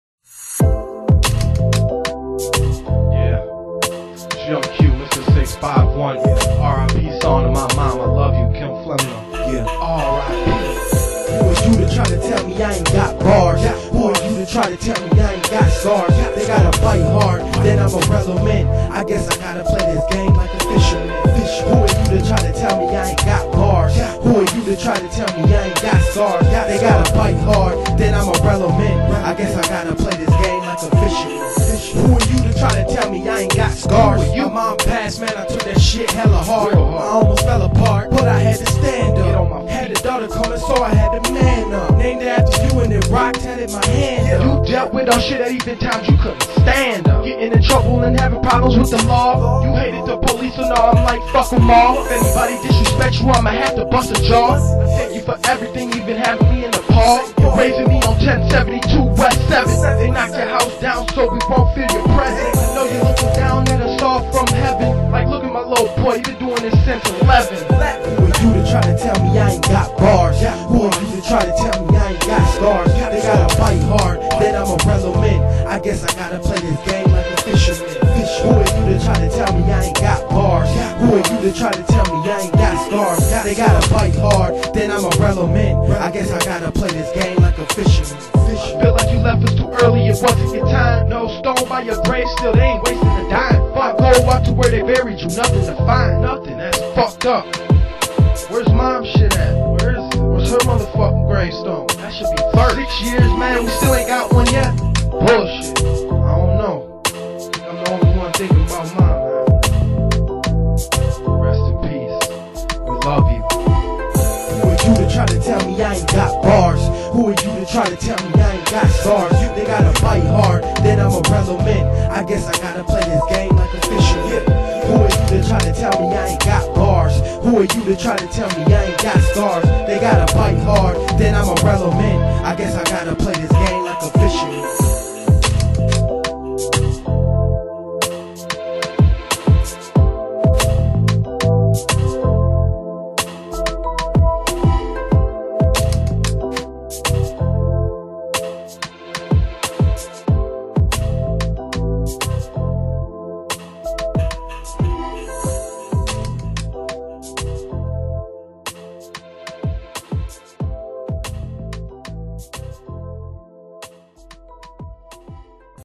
On-Q Tribute to Kim Flemino
This is a RIP Tribute to my Mom KIM FLEMINO who died from breast cancer in 2006 RIP WE LOVE N MISS U EVERY MIN for the ppl that have mothers. Your lucky appreciate her to the fullest ON-Q for freedownloads